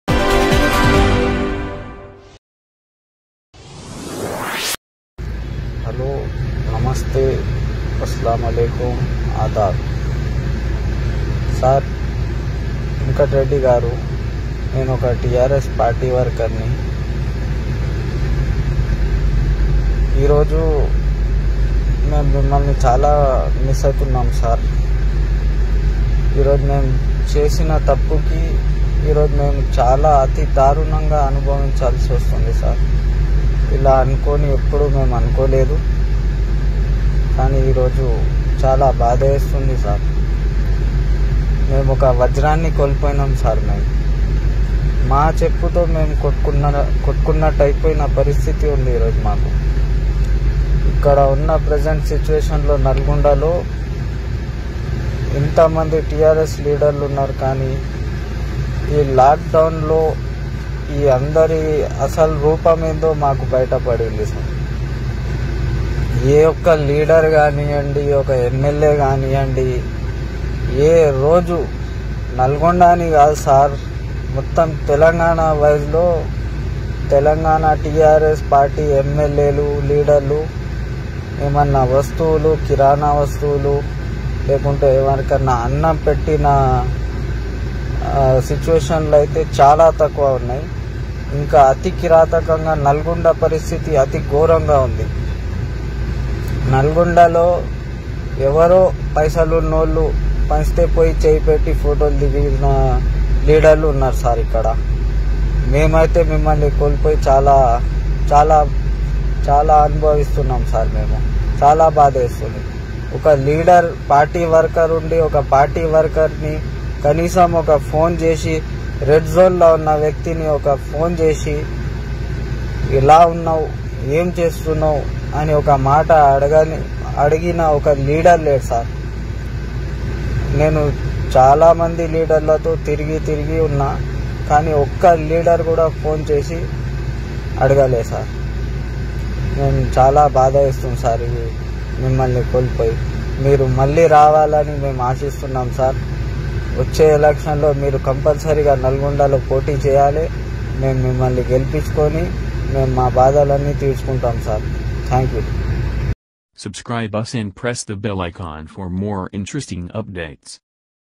కోమటిరెడ్డి ఓడించి తప్పుచేశాం..టీఆర్ఎస్ కార్యకర్త మాటలు వినండి|| Nalgonda Trs Worker On Komatireddy
Nalgonda Trs Worker On Komatireddy Venkat Reddy